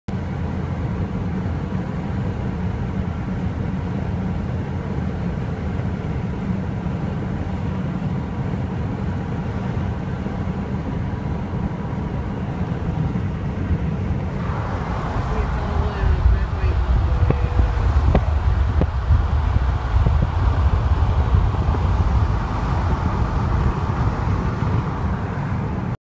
Basketball Hall of Fame lit up
Wicked early July 12th after I dropped Johnny and his mom at the airport.

I had mentioned during part 5 of my guest hosting about it lighting up at night and drove by wicked early the next morning and filmed it as I was going by on the highway.